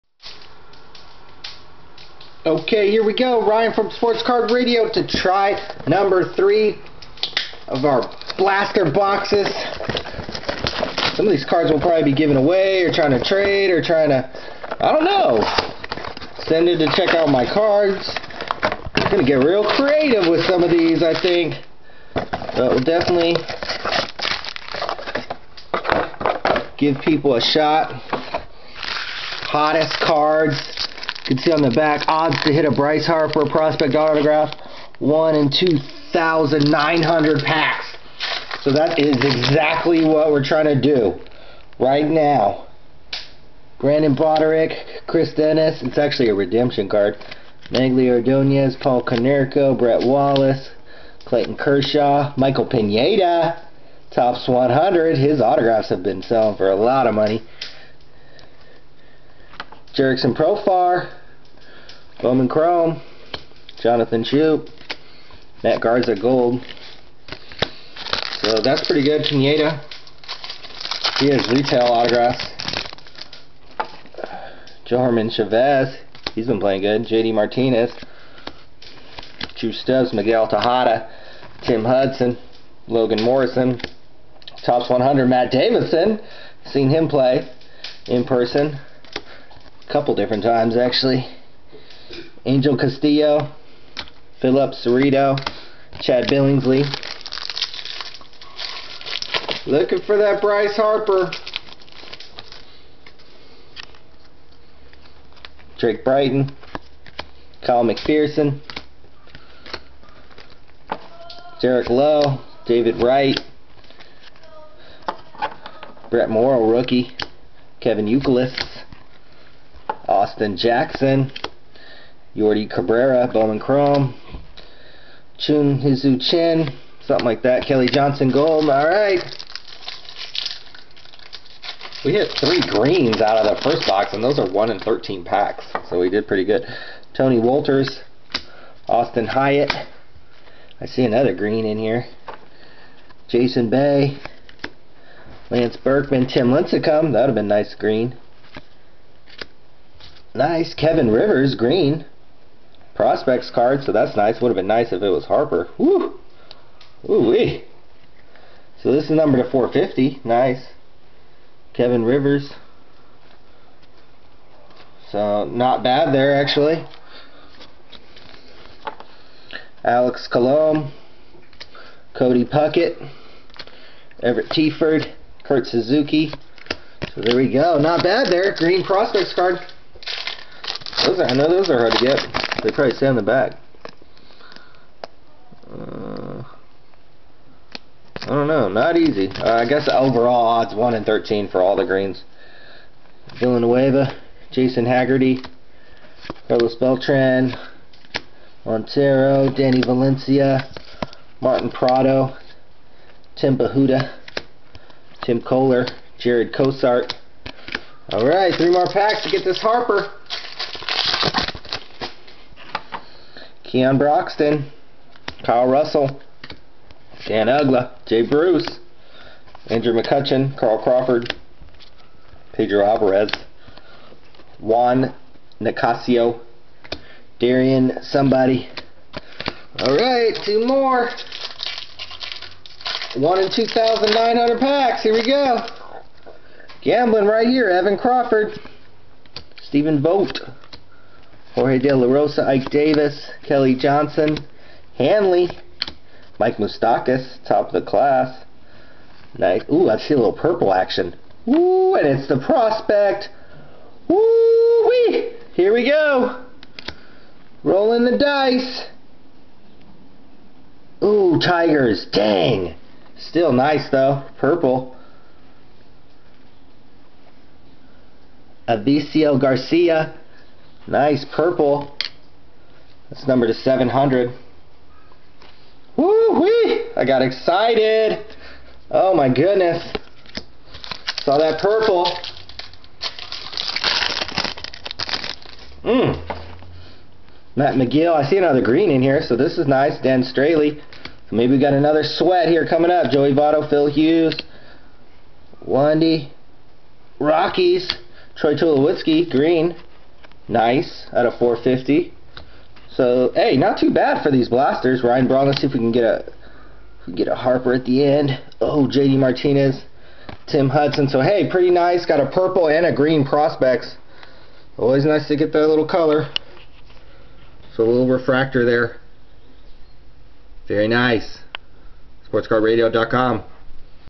2011 Bowman Baseball Retail Blaster Box Break 3
Round #3 of our 2011 Bowman Baseball Retail Box Breaks. Nice Purple Chrome Refractor! Check out a review and checklist of the set